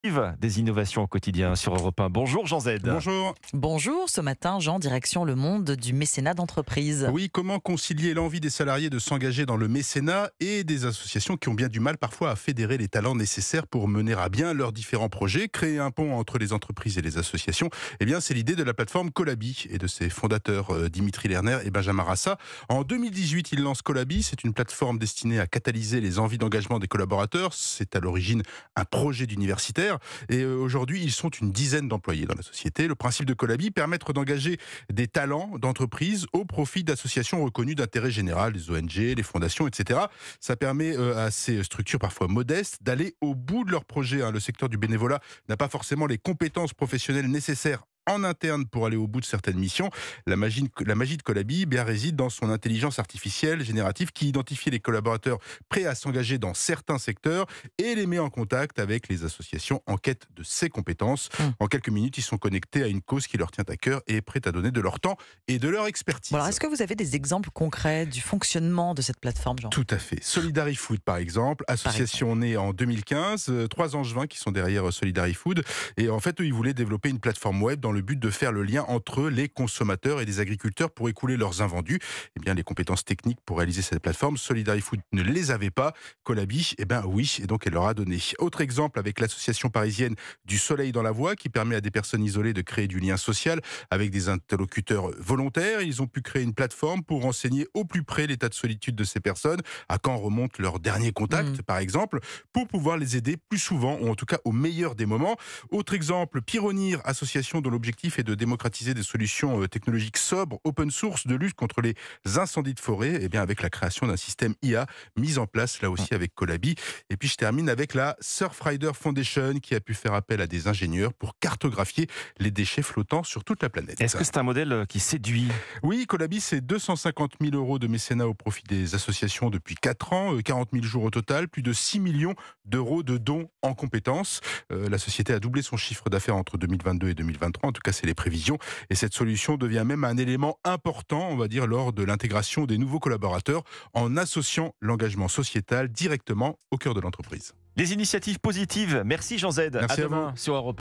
Quand les salariés se lancent dans le mécénat d’entreprise
Chaque matin, Jean Zeid livre ce qu’il se fait de mieux en matière d'innovation. Ce mardi, il revient sur le monde du mécénat d’entreprise.